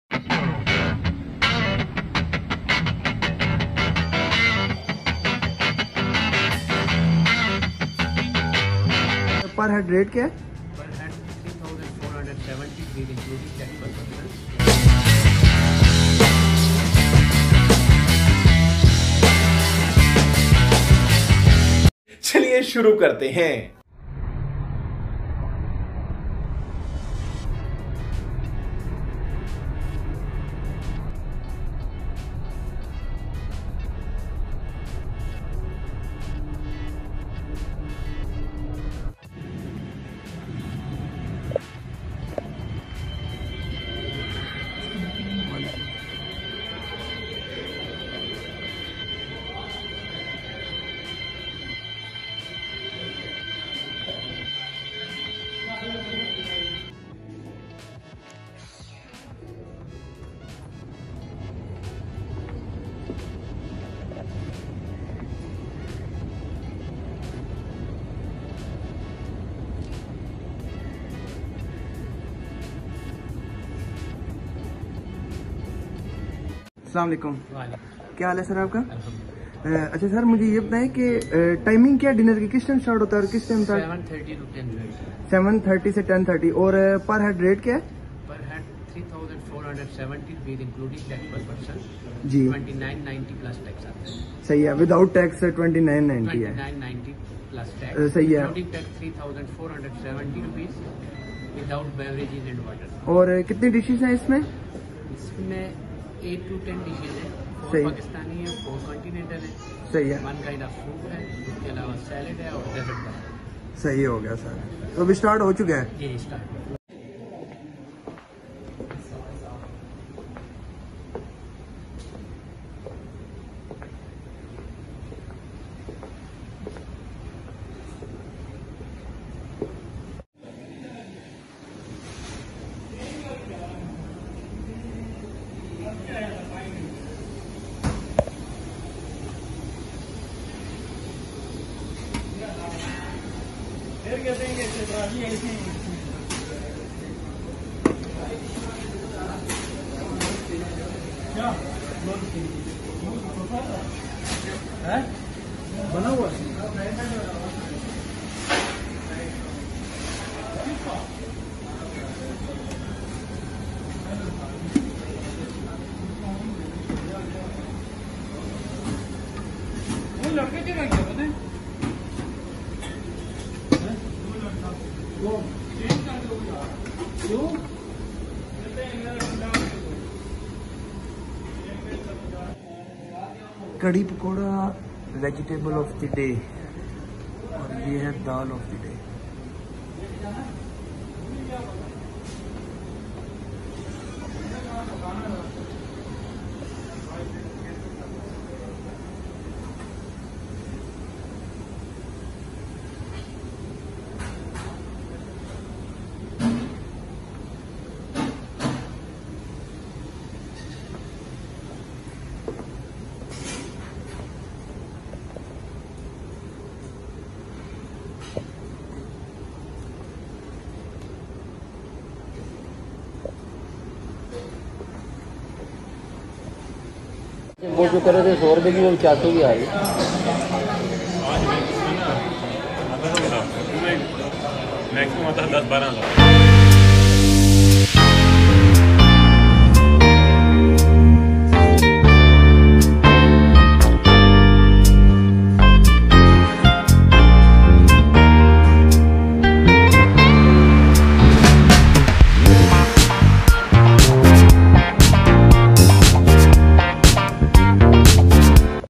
Buffet Dinner | Pearl Continental Hotel PC Rawalpindi | Marco Polo | 3470/- Rps Per Head | Not Rec
This Week I will teach Features Pearl Continental Hotel Rawalpindi which is pakistan First Five Star hotel.